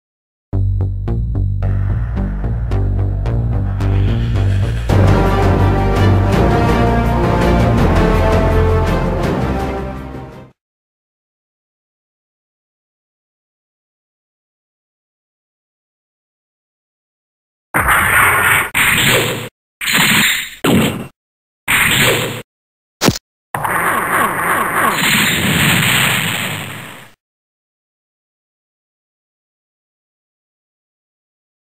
Gundam Stop Motion GM QUEL
Another stop motion test video. Tried to make it feel as f the mech was actually alive so i added a lot f sound effects to make it sound and look more realistic. Hope you like it!